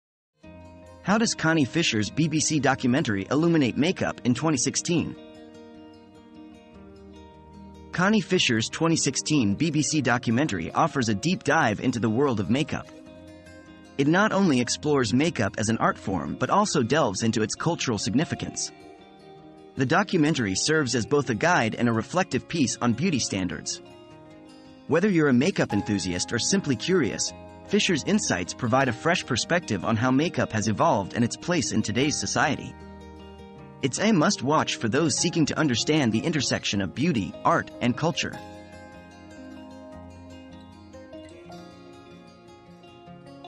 How Does Connie Fisher's BBC Documentary Illuminate Makeup in 2016?
Unveiling the Power of Makeup: Connie Fisher's BBC Documentary • The Power of Makeup • Discover the captivating world of makeup in Connie Fisher's 2016 BBC Documentary. Explore the artistry, cultural significance, and evolving beauty standards through Fisher's insightful lens. This must-watch film offers a fresh perspective on the intersection of beauty, art, and culture in today's society. Join us on this illuminating journey into the power of makeup.